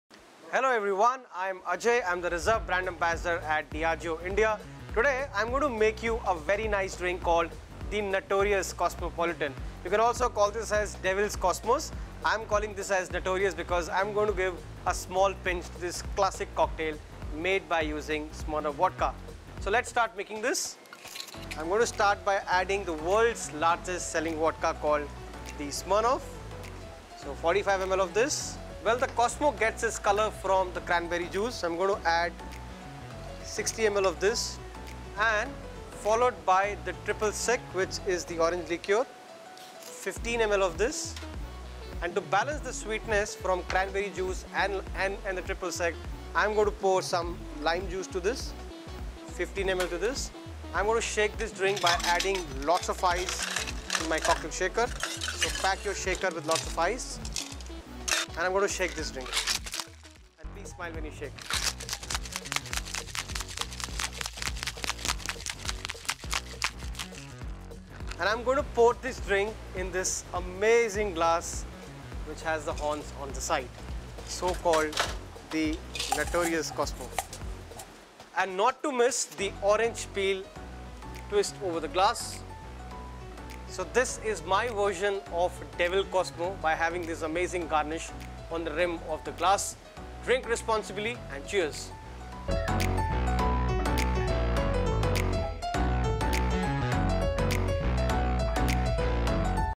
Easy Vodka Cocktail Recipe | Ajay Nayyar, Diageo Brand Ambassador
It’s not every day that a Diageo Reserve Brand Ambassador mixes a Negroni for you. But then Negroni Week is special and Ajay Nayyar was happy to oblige